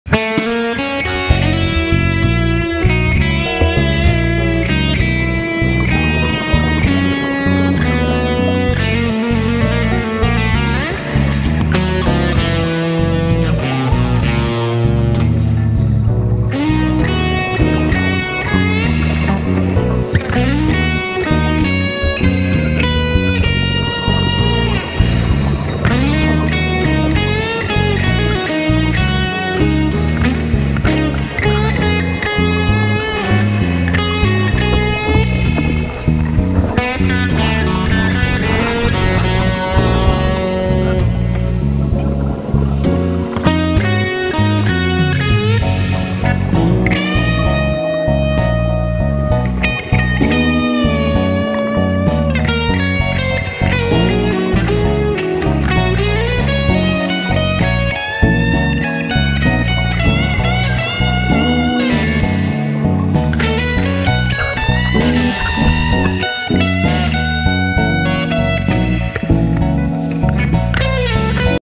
sunken boat under lake travis texas
Scuba Diving Lake Travis Texas found sunken boat